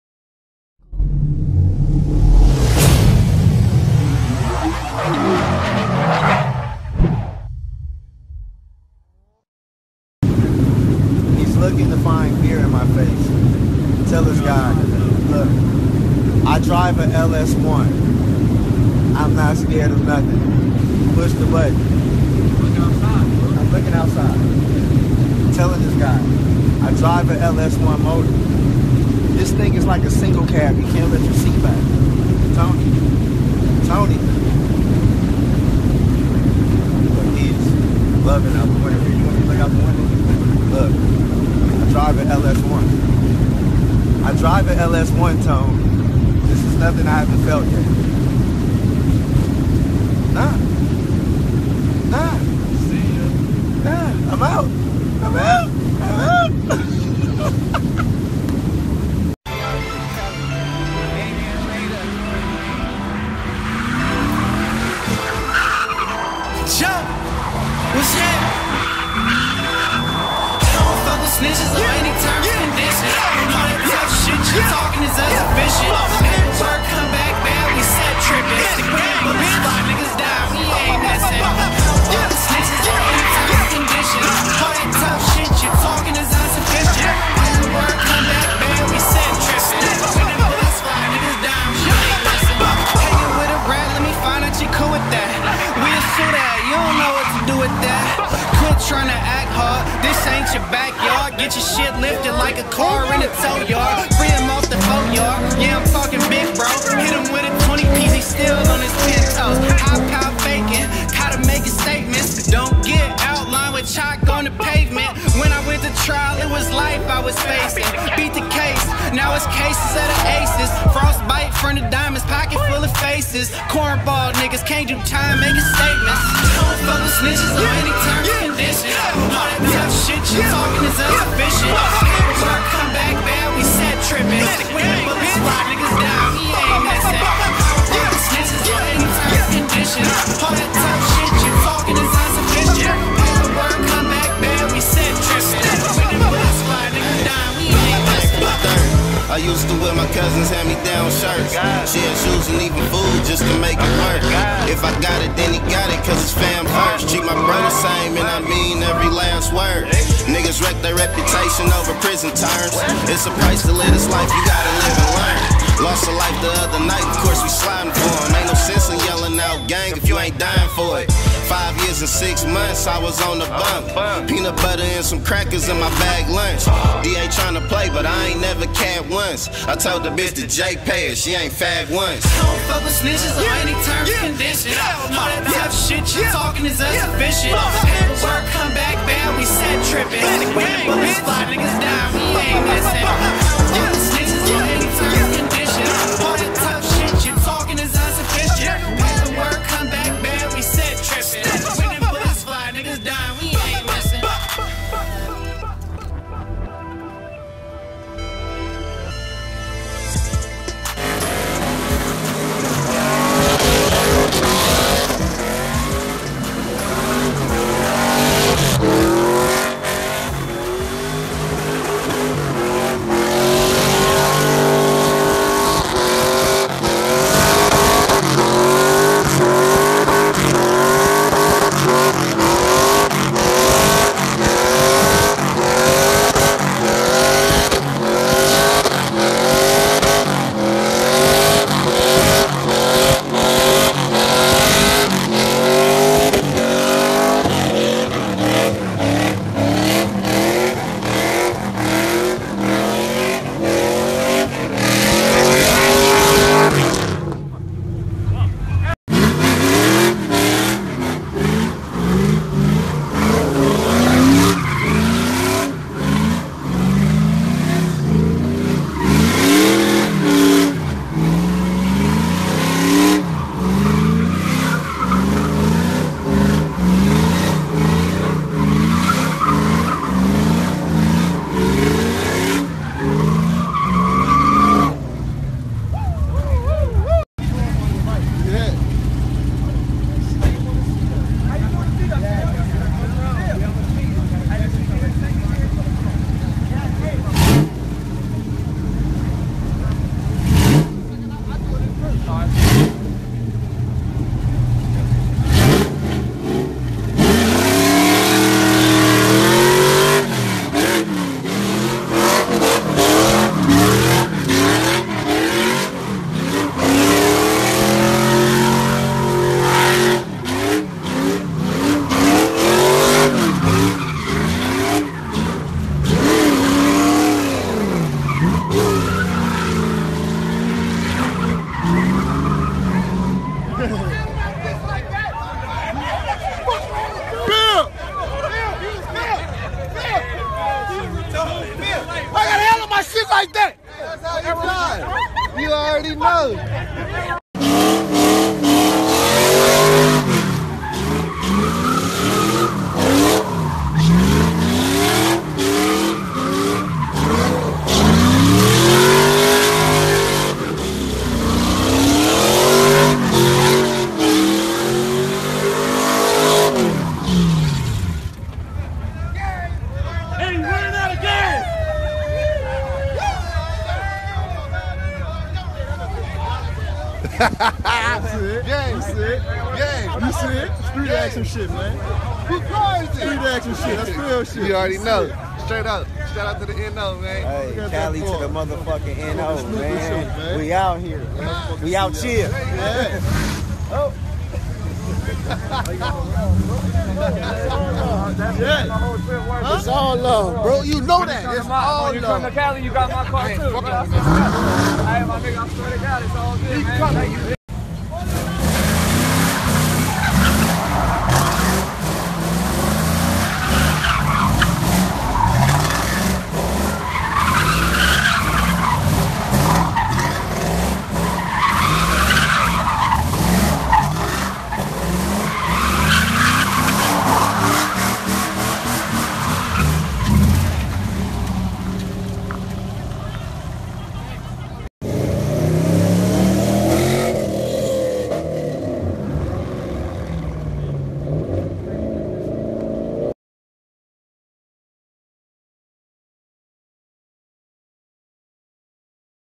FASTLYFE EASTSIDE TV | CALI 2 NOLA | New Orleans takeover | BURNOUT COMPILATION
Cali 2 NOLA put on by East Side TV. burnout compilation the boys are putting it down on New Orleans. the bay area movement is real

Video by